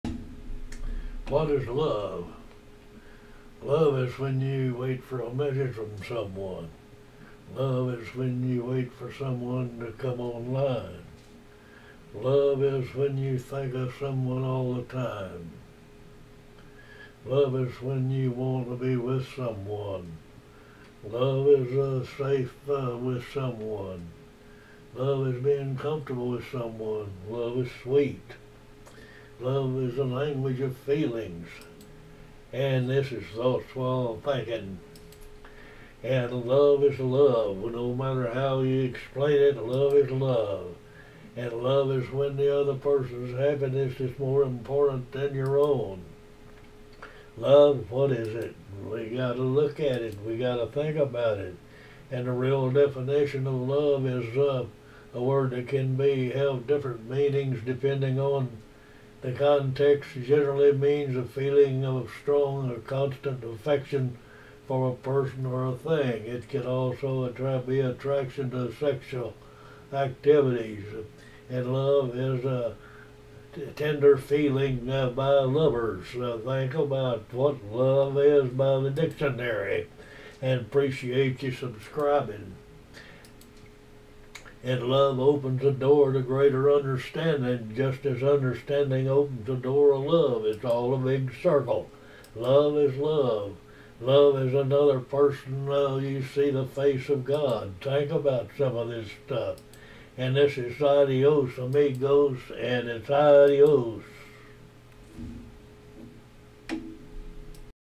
The influence or force that motivates the life of each soul is love
what Is Love and How Does It Affect Us?
Thoughts While Thinking? It is our interpretation of what is the bigger picture and connecting the dots of what is not seen on the surface and putting it into a simple scenario using the experience oof covering news. Bill is a retired owner of Radio and TV stations and a Past President of the Texas Association of Broadcaster’s
I’m The Oldest Active You tubber!!